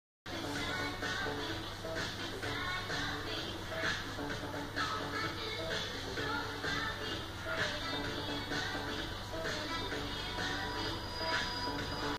Front Lever 10 seconds
Doing some specific training drills helps to develop a needed skill as soon as you have a decent foundational strength.
10 seconds Front Lever. Aim is 20 sec.